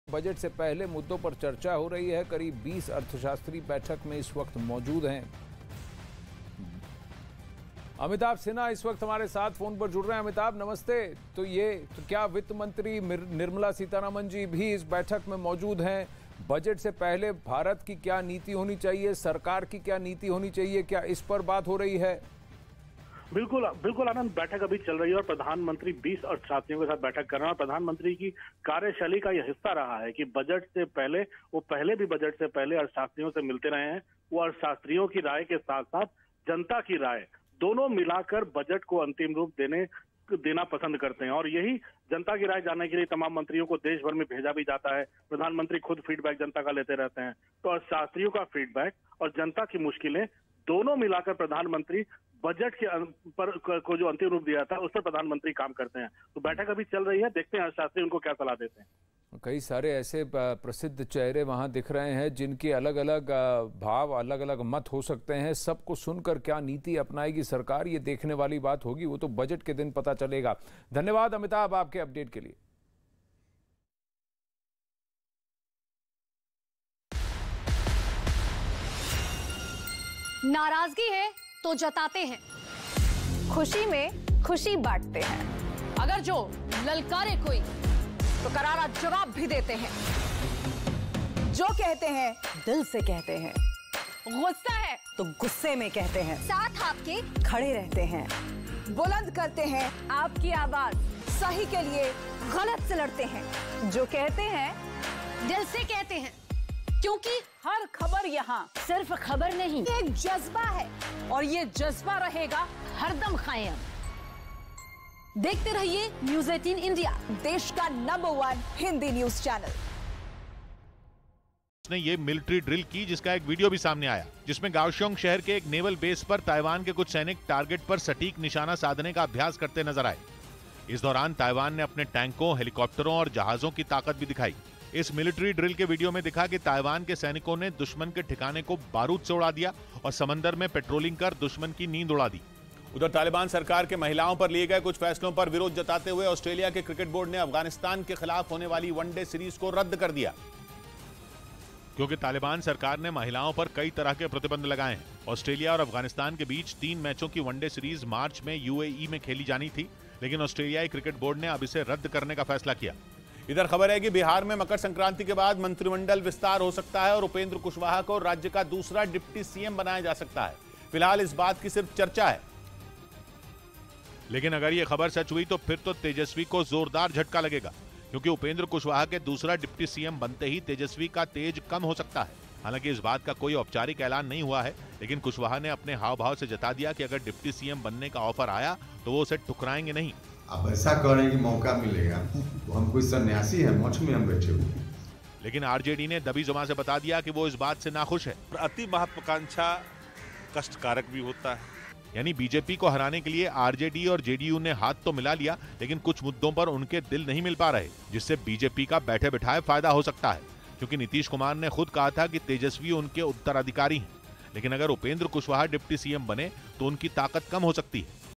Breaking News: PM Modi की बज़ट से पहले अर्थशास्त्रियों के साथ बैठक। Latest Hindi News। Budget 2023
Breaking News:  PM Modi आज नीति आयोग में अर्थशास्त्रियों के साथ की। इस बैठक के दौरान वित्त मंत्री निर्मला सीतारमण भी मौजूद रहीं। पीएम ने केंद्रीय बजट से पहले अर्थशास्त्रियों से राय और सुझाव लेने के साथ-साथ भारतीय अर्थव्यवस्था की स्थिति और इसकी चुनौतियों का आकलन किया

Breaking News: PM Modi interacted with economists in NITI Aayog today. Finance Minister Nirmala Sitharaman was also present during this meeting. PM assesses the state of the Indian economy and its challenges, taking views and suggestions from economists ahead of the Union Budget.